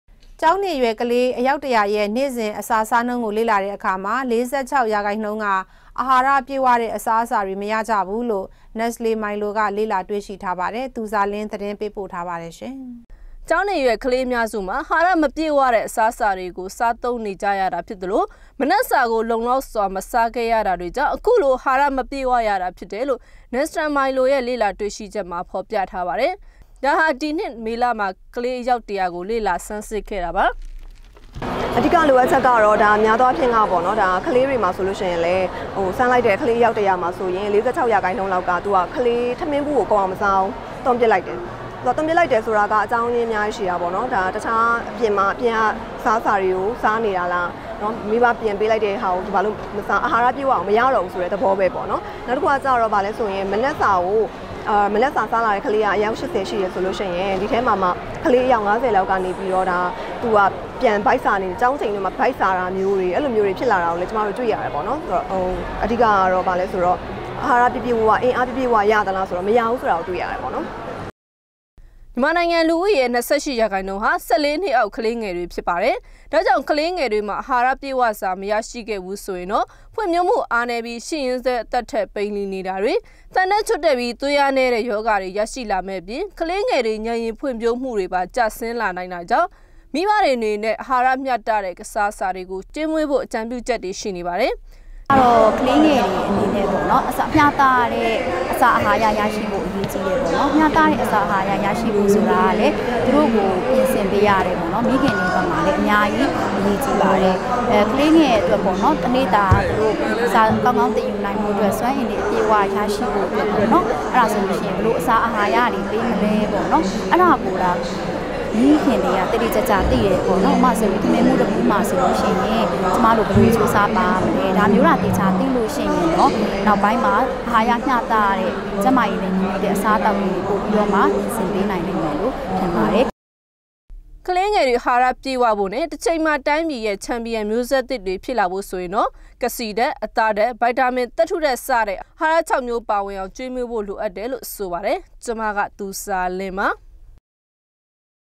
DVB - ေက်ာင္းေနရြယ္ ကေလး ၄၆ % အဟာာရျပည့္ဝတဲ့ အစားအစာေတြ မရၾက
ေက်ာင္းေနအရြယ္ ကေလးအေယာက္ ၁၀၀ ရဲ႕ ေန႔စဥ္ အစာ စားႏႈန္းကို ေလ့လာတဲ့အခါမွာ ၄၆ ရာခုိင္ႏႈန္းက အဟာာရျပည့္ဝတဲ့ အစားအစာေတြ မရၾကဘူးလို႔  Nestle Milo က ေလ့လာေတြ႔ရွိထားပါတယ္။  သူဇာလင္းသတင္းေပးပို႕ထားပါတယ္။